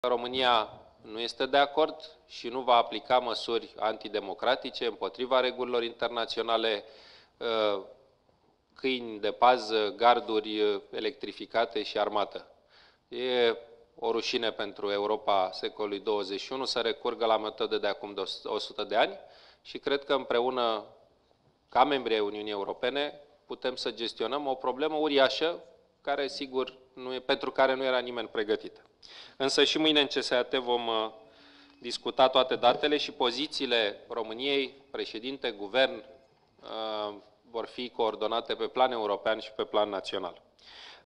Ședința Guvernului României din 16 septembrie 2015 (imagini protocolare)
Victor Ponta: „România nu este de acord și nu va aplica măsuri antidemocratice împotriva regulilelor internaționale, câini de pază, garduri electrificate și armată”
Titlul evenimentului: Ședința Guvernului României din 16 septembrie 2015 (imagini protocolare)